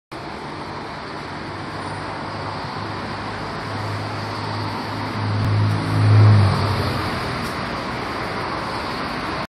Audi A8 4.2 V8 n Dog Walking 20131106_070919.mp4
Last gen Audi A8L being driven past an senior dog walking . Share , like , sub for more luxury motor car video's .